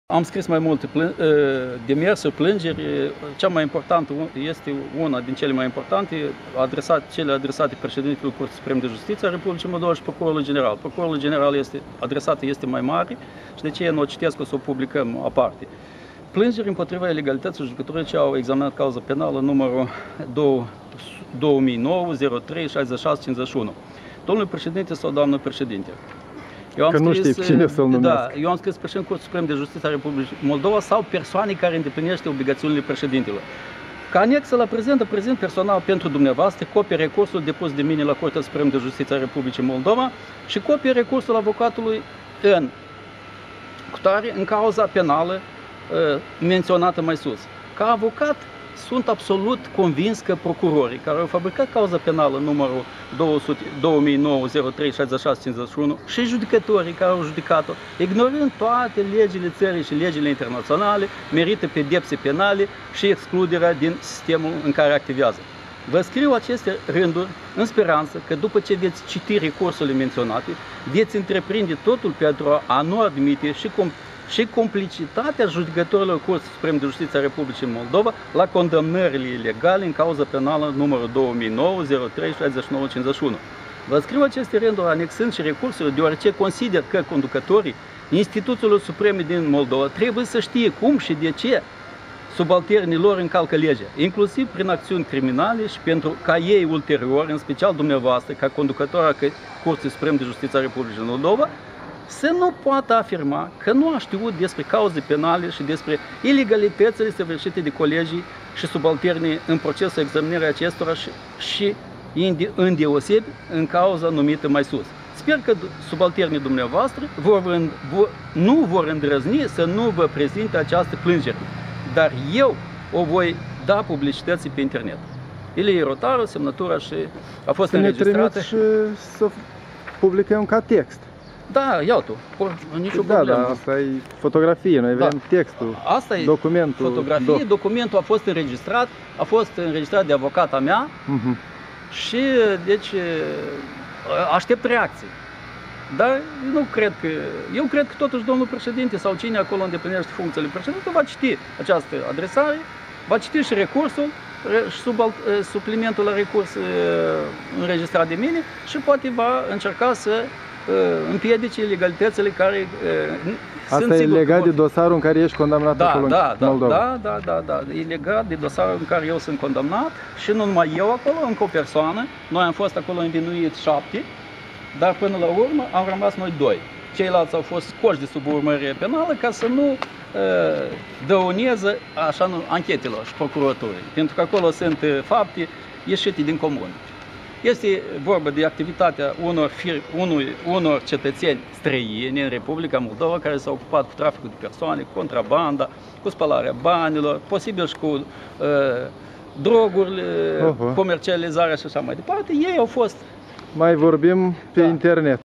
A scris plîngere preşedintelui CSJ - Curaj.TV
Avocatul Ilie Rotaru a trimis depeşă din România către conducerea Curţii Supreme de Justiţie din Moldova, acolo se află pe rol dosarul penal în care a fost condamnat în primele două instanţe şi spune că e vorba de răfuieli politice.
Mărturie de la Bucureşti, din preajma Parchetului General